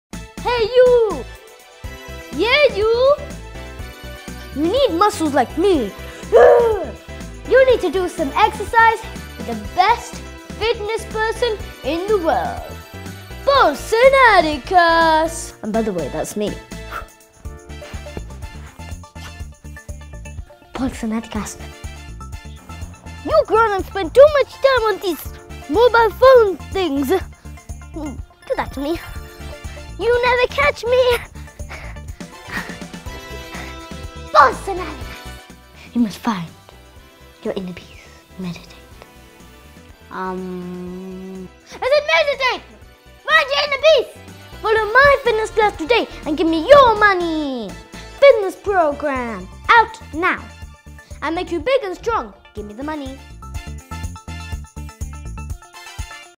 Introducing Polson Adikaas the fitness motivator
Are you always on your mobile phone? Do you want to be big and strong?
Well you need Polson Adikaas in your life. Join the fitness and life coach as he shows you how to become a better human being.